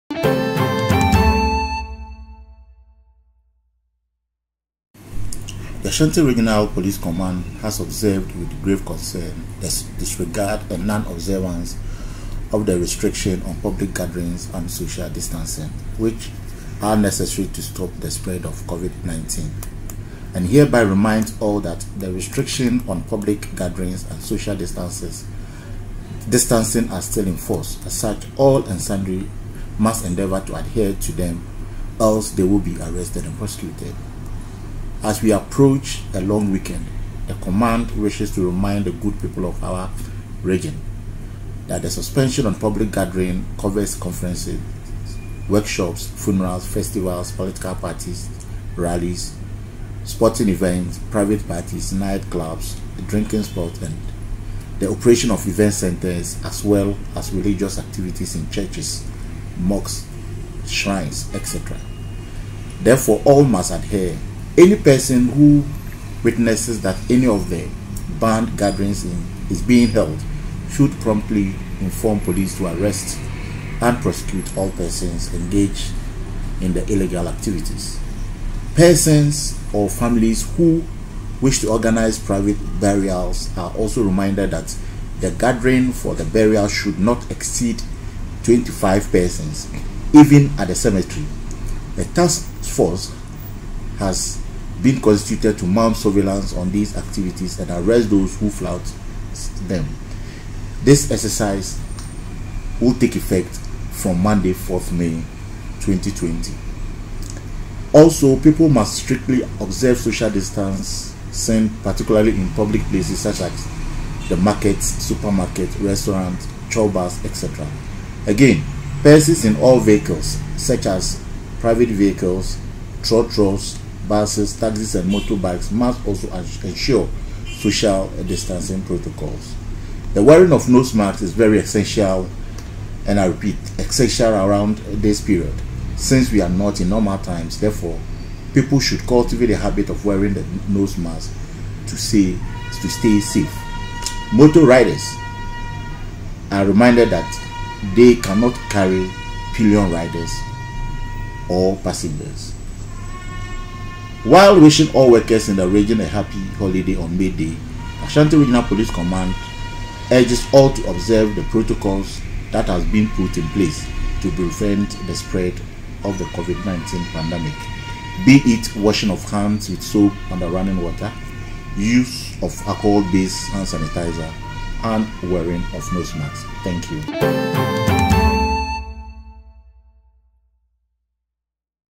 Ash. Regional Police Command Education And Enforcement Of The Restriction Acts
The Ashanti Regional Police Command has reminded the public that the restriction on public gathering and social distancing is still in force therefore people should take note and must endeavor to adhere to them else face arrest and prosecution.